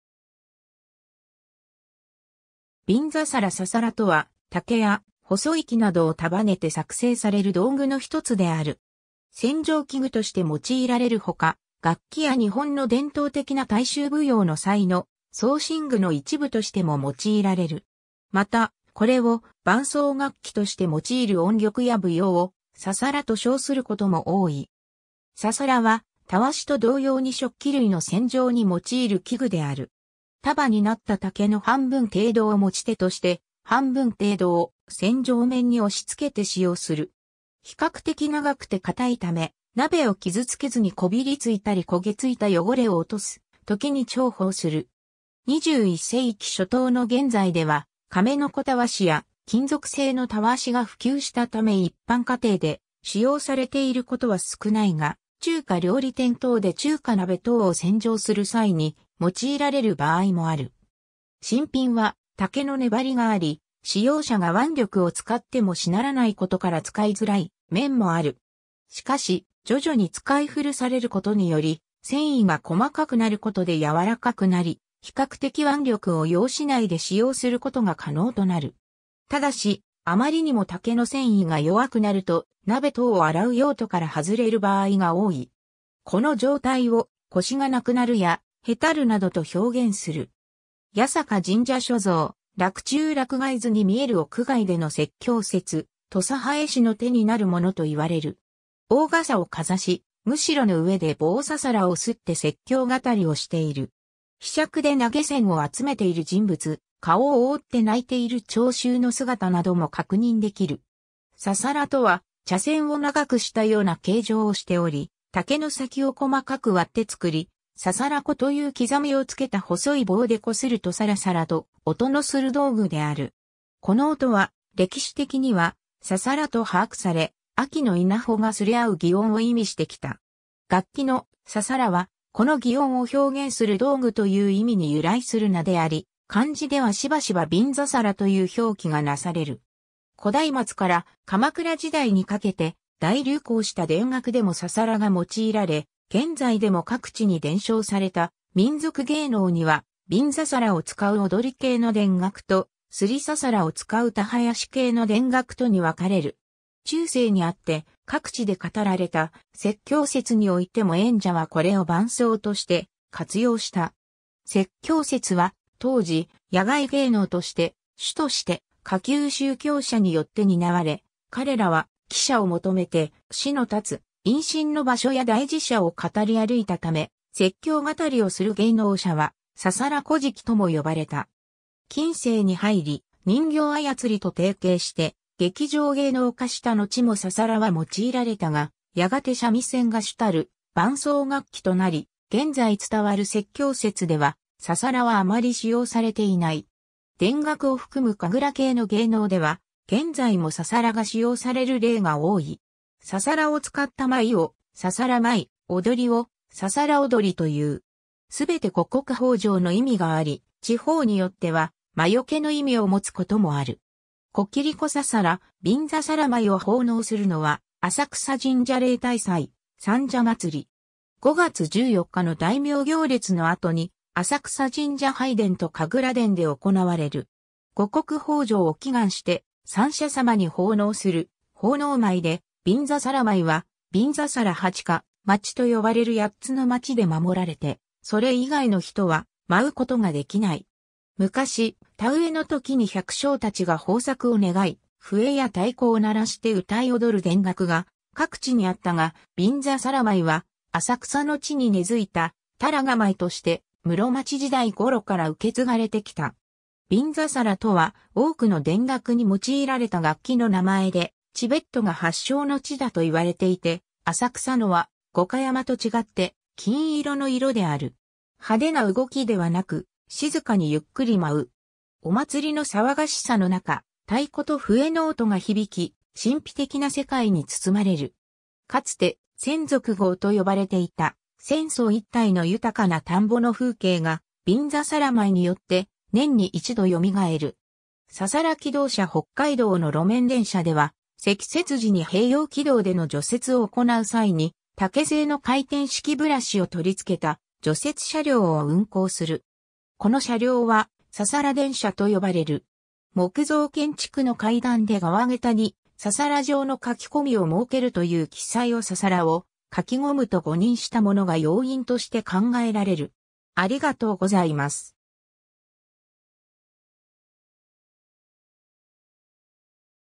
ささら
びんざさら（こきりこささら） ささら（簓）とは、竹や細い木などを束ねて作製される道具の一つである。
洗浄器具として用いられるほか、楽器や日本の伝統的な大衆舞踊の際の装身具の一部としても用いられる。
また、これを伴奏楽器として用いる音曲や舞踊を「ささら」と称することも多い。
 ささらは、たわしと同様に食器類の洗浄に用いる器具である。
 束になった竹の半分程度を持ち手として半分程度を洗浄面に押しつけて使用する。
比較的長くて硬いため、鍋を傷つけずにこびりついたり焦げついた汚れを落とすときに重宝する。
21世紀初頭の現在では亀の子たわしや金属製のたわしが普及したため一般家庭で使用されていることは少ないが、中華料理店等で中華鍋等を洗浄する際に用いられる場合もある。
 新品は竹の粘りがあり、使用者が腕力を使ってもしならないことから使いづらい面もある。
しかし、徐々に使い古されることにより繊維が細かくなることで柔らかくなり比較的腕力を要しないで使用することが可能となる。
ただし、あまりにも竹の繊維が弱くなると鍋等を洗う用途から外れる場合が多い。
この状態を「腰がなくなる」や「へたる」などと表現する。
 八坂神社所蔵『洛中洛外図』（元和年間）にみえる屋外での説経節 土佐派絵師の手になるものといわれる。
大傘をかざし、むしろの上で棒ささらを摺って説経語りをしている。
ひしゃくで投げ銭を集めている人物、顔をおおって泣いている聴衆の姿なども確認できる。
 ささらとは、茶筅を長くしたような形状をしており、竹の先を細かく割ってつくり、「ささら子」という刻みをつけた細い棒でこするとサラサラと音のする道具である。
この音は、歴史的には「ささら」と把握され、秋の稲穂が擦れあう擬音を意味してきた。
楽器の「ささら」は、この擬音を表現する道具という意味に由来する名であり、漢字ではしばしば編木という表記がなされる。
 古代末から鎌倉時代にかけて大流行した田楽でもささらが用いられ、現在でも各地に伝承された民俗芸能には、びんざさらを使う躍り系の田楽と、擦（す）りささらを使う田囃子（はやし）系の田楽とに分かれる。
 中世にあって、各地で語られた説経節においても演者はこれを伴奏として活用した。
説経節は当時、野外芸能（大道芸・門付芸）として、主として下級宗教者によって担われ、かれらは喜捨を求めて市の立つ殷賑の場所や大寺社を語り歩いたため、説経語りをする芸能者は「ささら乞食」とも呼ばれた。
近世に入り、人形操りと提携して劇場芸能化したのちもささらは用いられたが、やがて三味線が主たる伴奏楽器となり、現在伝わる説経節では、ささらはあまり使用されていない。
 田楽を含む神楽系の芸能では、現在もささらが使用される例が多い。
ささらを使った舞をささら舞、踊りをささら踊りという。
全て五穀豊穣の意味があり、地方によっては魔よけの意味をもつこともある。
 こきりこささら（びんざさら） 「びんざさら舞」を奉納するのは浅草神社（三社様）例大祭・三社祭。
5月14日の大名行列の後に浅草神社拝殿と神楽殿で行われる。
五穀豊穣を祈願して、三社様に奉納する奉納舞で、「びんざさら舞」は『びんざさら八か町』と呼ばれる八つの町で守られて、それ以外の人は舞うことができない。
昔、田植えの時に百姓たちが豊作を願い、笛や太鼓を鳴らして歌い踊る田楽が各地にあったが、「びんざさら舞」は浅草の地に根付いた田楽舞として、室町時代ごろから受け継がれてきた。
「びんざさら」とは、多くの田楽に用いられた楽器の名前で、チベットが発祥の地だと言われていて、浅草のは五箇山と違って金色の色である。
派手な動きではなく、静かにゆっくり舞う。
お祭りの騒がしさの中、太鼓と笛の音が響き、神秘的な世界に包まれる。
かつて「千束郷」と呼ばれていた浅草一体の豊かな田んぼの風景が「びんざさら舞」によって、年に一度よみがえる。
 ササラ気動車 北海道の路面電車（2014年現在運用されているのは札幌市電と函館市電）では積雪時に併用軌道での除雪を行う際に竹製の回転式ブラシを取り付けた除雪車両を運行する。
この車両は「ササラ電車」と呼ばれる。
 ↑木造建築の階段で側桁にササラ状の欠き込みを設けるという記載をササラを欠き込むと誤認したものが要因として考えられる。